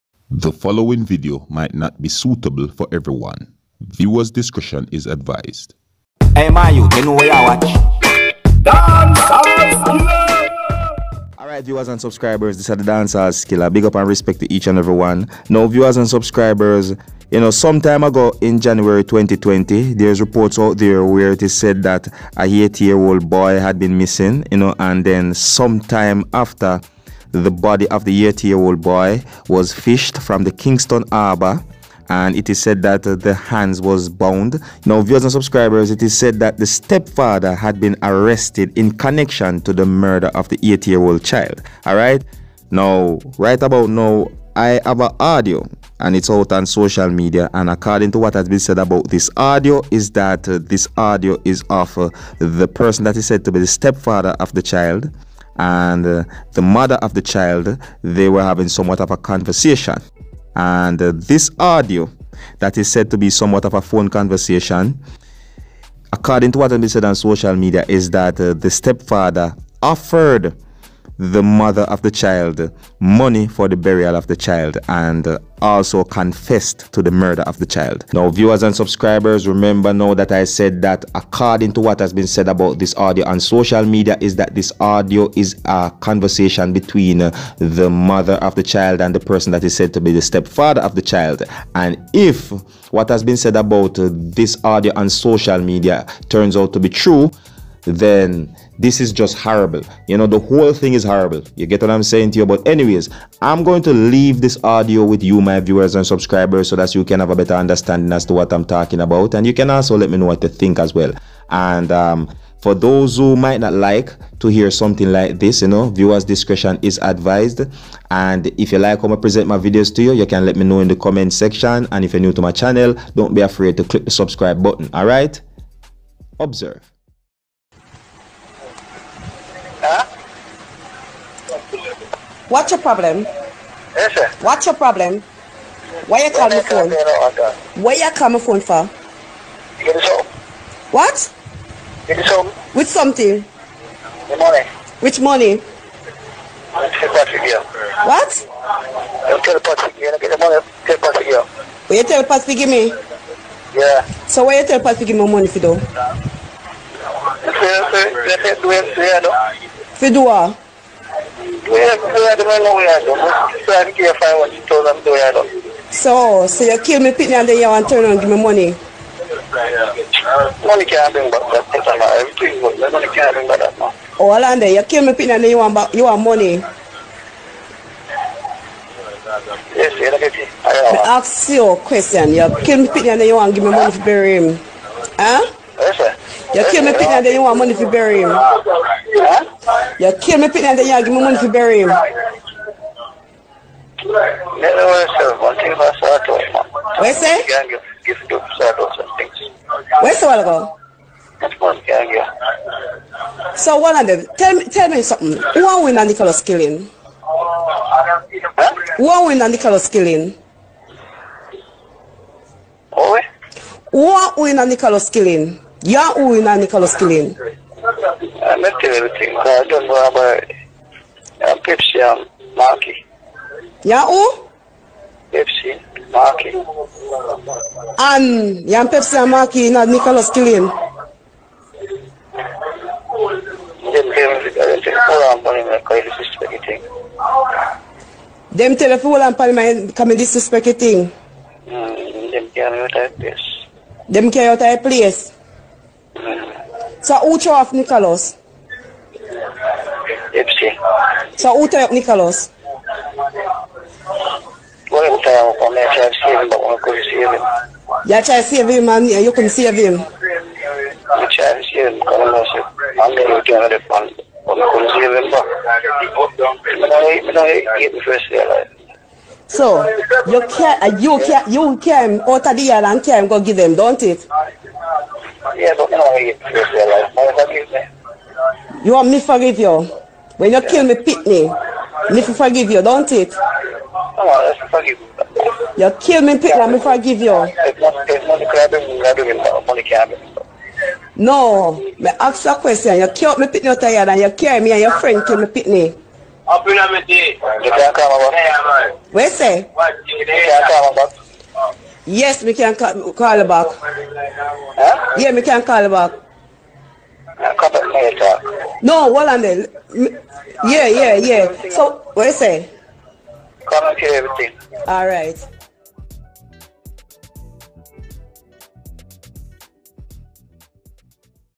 Stepfather accused of K!££!NG 8 yrs old confess n offered money for funeral in phone conversation
WARNING !
Rated - PG !